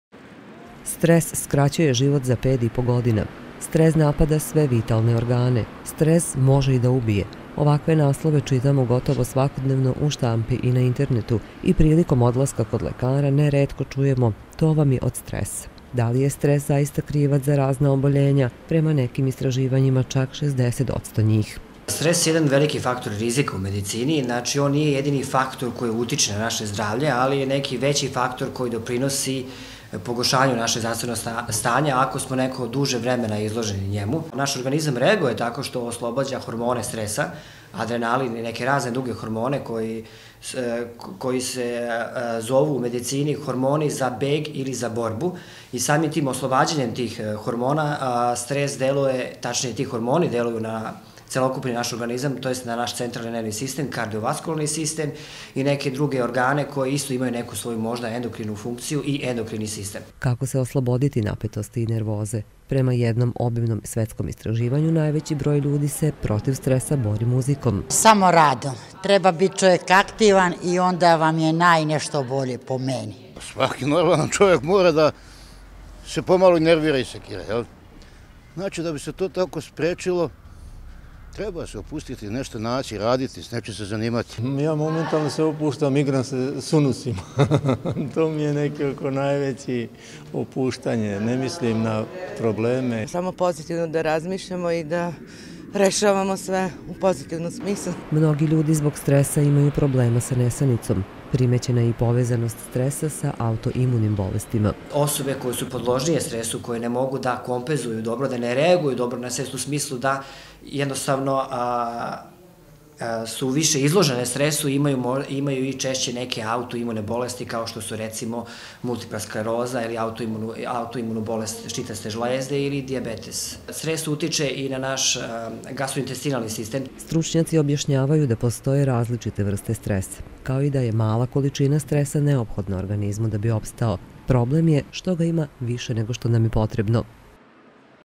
Stres
Američka zdravstvena asocijacija zaključila je da je stres uzrok čak 60 odsto svih bolesti.  Svakodnevno slušamo kako je baš on krivac za stanje u kome se nalazi naše fizičko i mentalno zdravlje, da stres umanjuje hormone štitne žlezde, ubrzava starenje, da utiče na mozak, srce, creva, gojaznost. Da li zaista možemo da pripišemo stresu nastanak oboljenja i kojih?